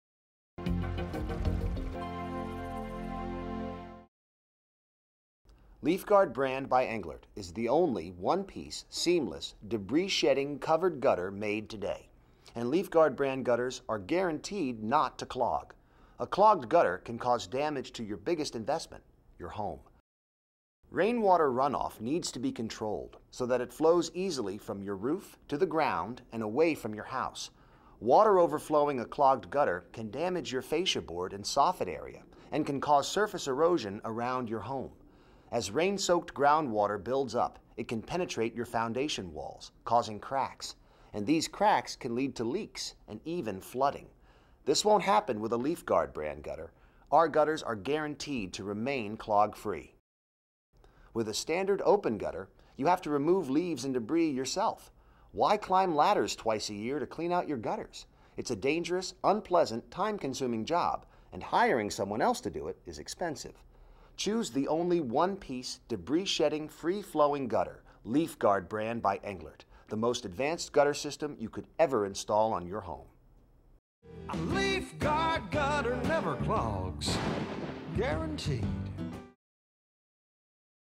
Why You Shouldn't Go Another Day Without LeafGuard Gutters
A clogged gutter can cause damage to your biggest investment-your home.  With LeafGuard Gutters, you'll never worry about clogged gutters again.  They're specially designed never to clog and if for any reason, they ever do, we'll come out and clean them free of charge.